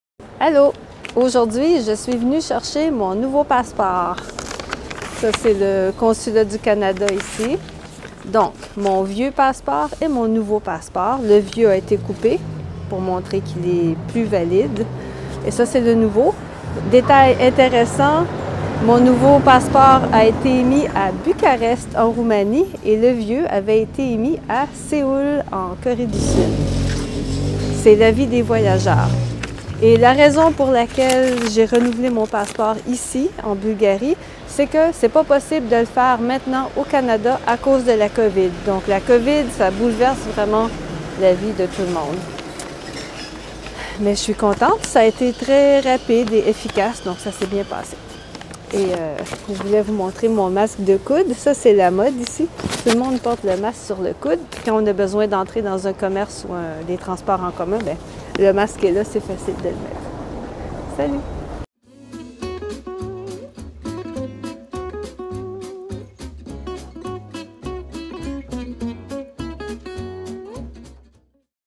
FRENCHPRESSO (Slow, Easy French) - Mon nouveau passeport
Avec la pandémie de la Covid-19, tout est plus difficile... Incluant la tâche de renouveler son passeport! Je vous raconte ça ici. C'est bizarre, mais ce n'était pas possible de renouveler mon passeport canadien au Canada... J'ai dû revenir ici, en Bulgarie, pour le renouveler! Étonnant, non?